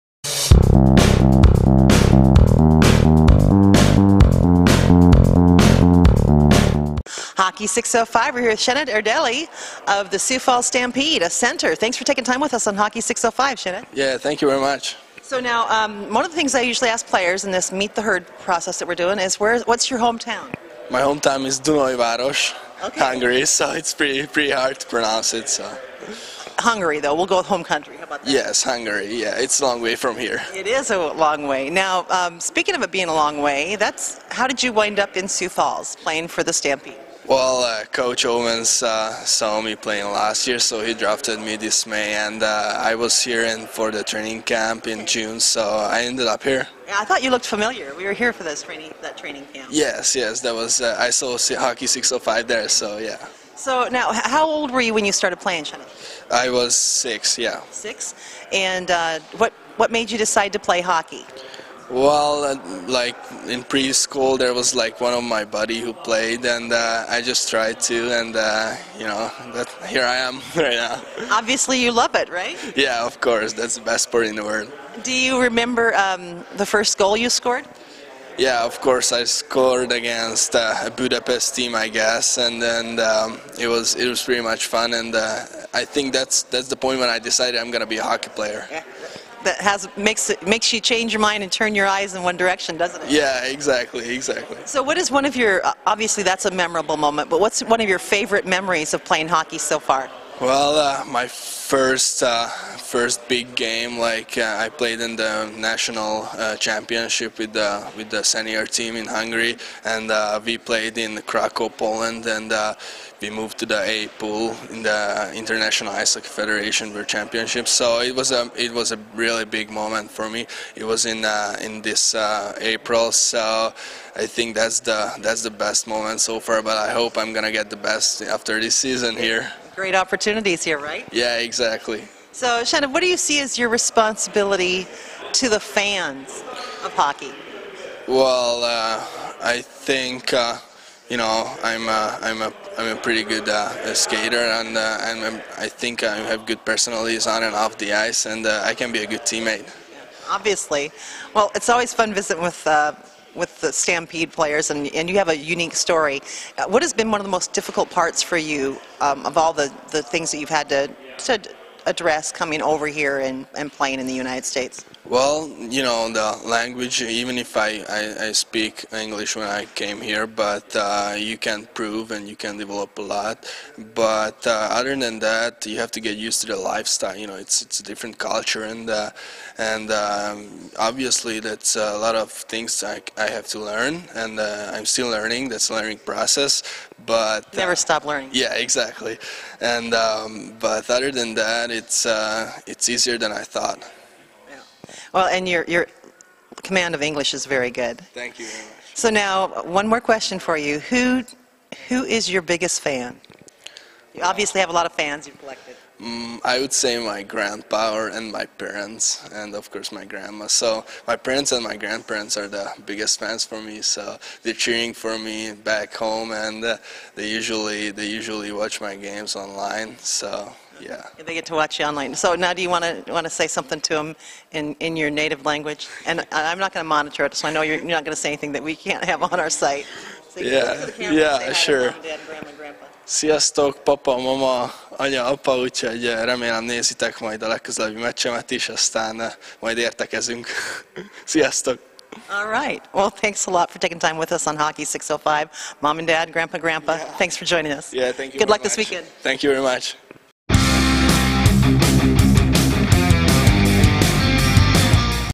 #13 Csanád Erdely @erdely36 of
Thanks for joining Hockey605, YOUR South Dakota hockey source.
October 29, 2015 at Scheels IcePlex, Sioux Falls, SD - Hockey605 visits with Sioux Falls Stampede forward Csanád Erdely, #13